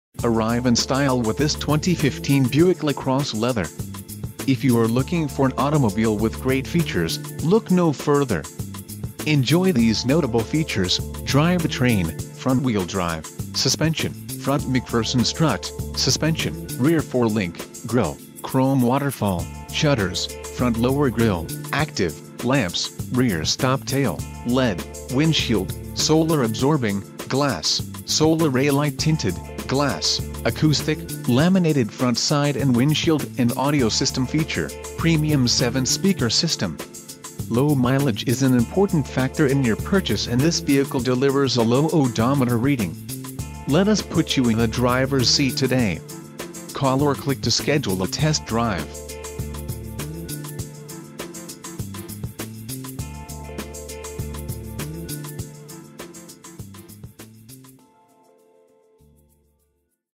2015 Buick LaCrosse Leather - Kelly Buick GMC - Emmaus, P...
For Sale in Emmaus, PA 18049

2015 Buick LaCrosse Leather Condition: New Mileage: 4 Transmission: Automatic Exterior Color: Deep Garnet Metallic Doors: 4 Stock: B52008 Cylinders: 6